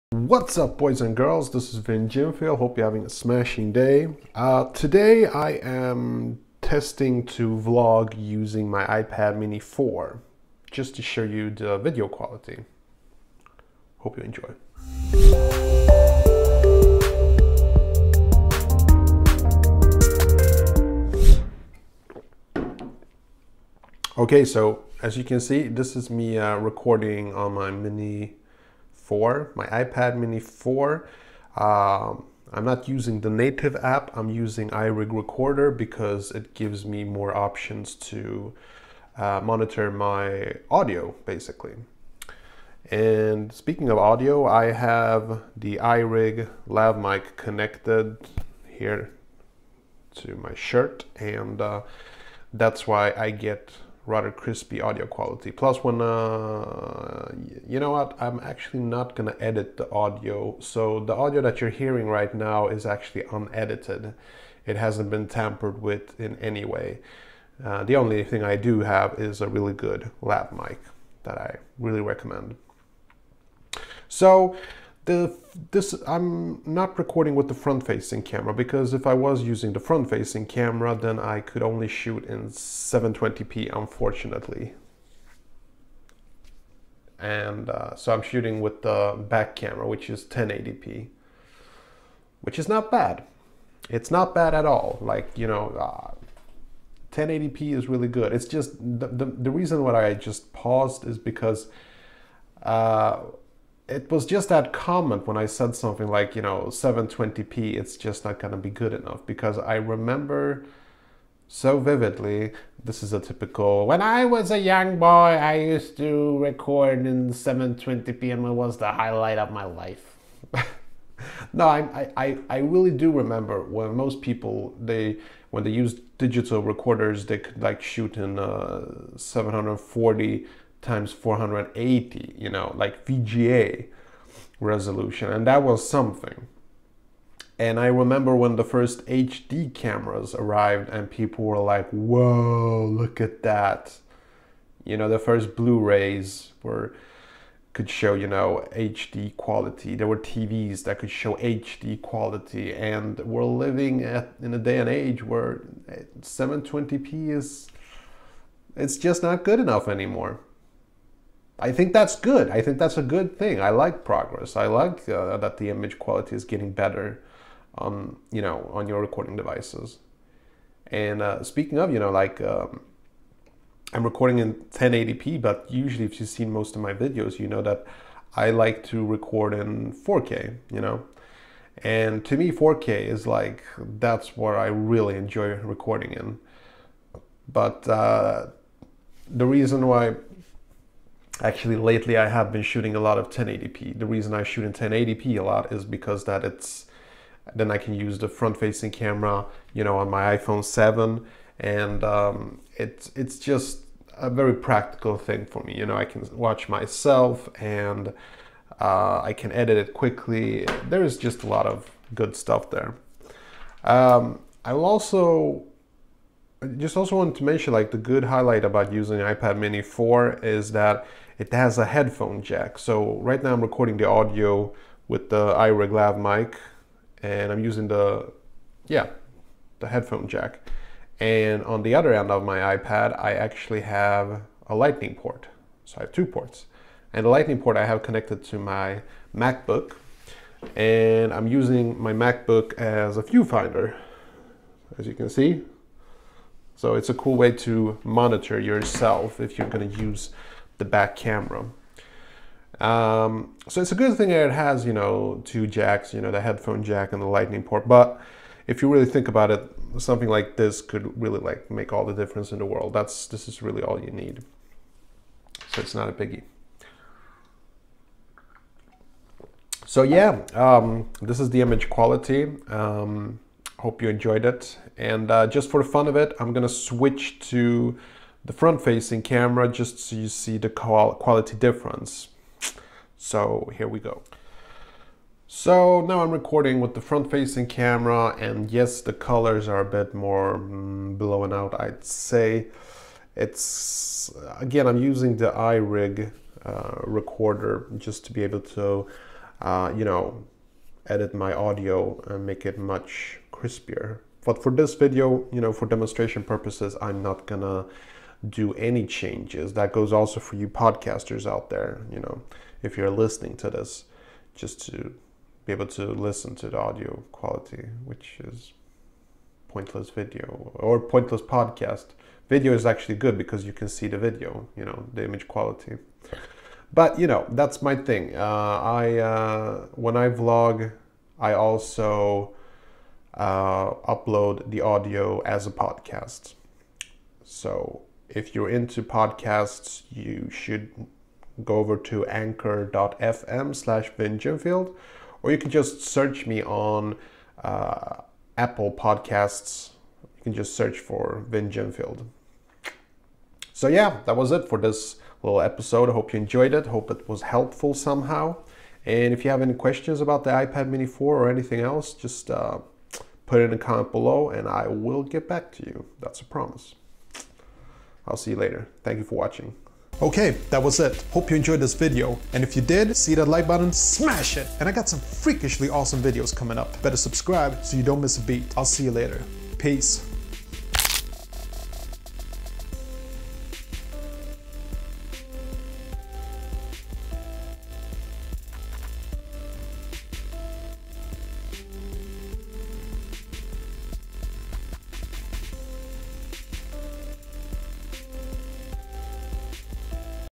📹 Ipad Mini 4 Video samples - How Does it Look When Vlogging with iPad mini 4 (front / back camera)
In this episode I talk about, well I show you what it looks like when I film with my ipad mini 4. sort of a sample video. enjoy.

NEW EPISODE IS UPLOADED EVERY DAY / SAME TIME:
New York(USA          12.00
Los Angeles(USA)    09.00
Tokyo(japan)             02.00
Sydney(Australia)     04.00
London(UK)               17.00
Stockholm(SE)          18.00